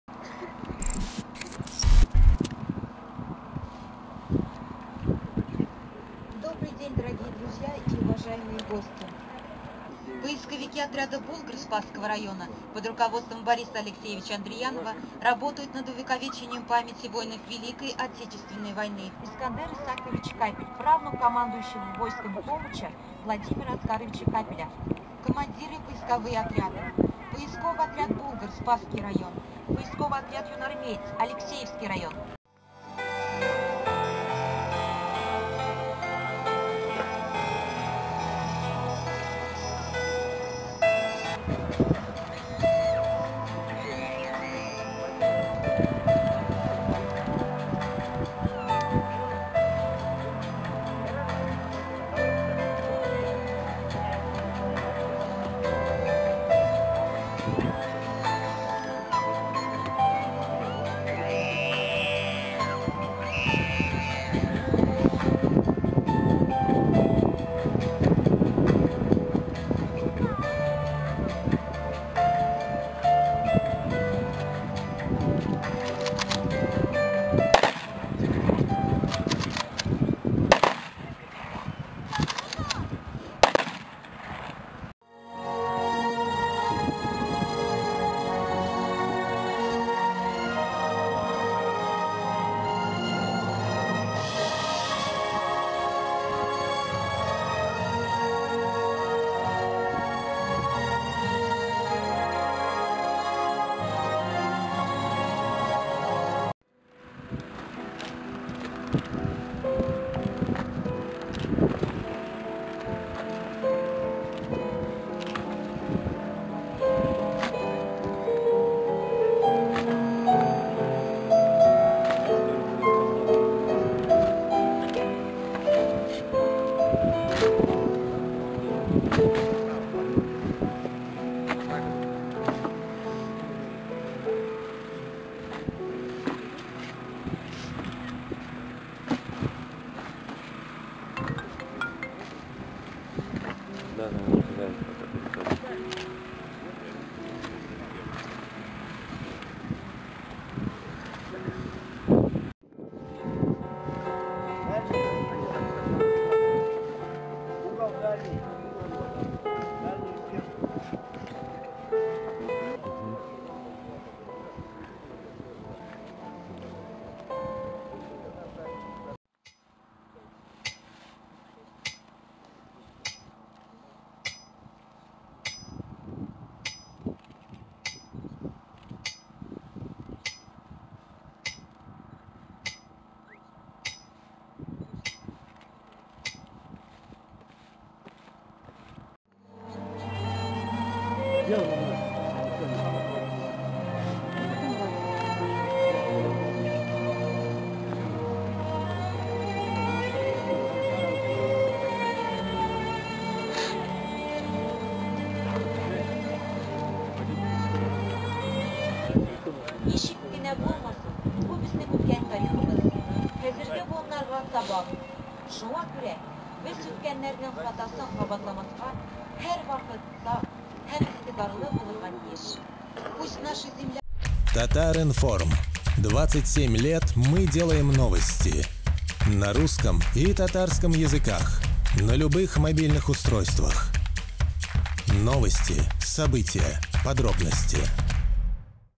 В Спасском районе РТ захоронили останки жертв гражданской войны 1918-1921гг
Республиканская «Вахта Примирения и Согласия» подарила покой еще 39-и воинам, погибшим в братоубийственной войне начала прошлого века.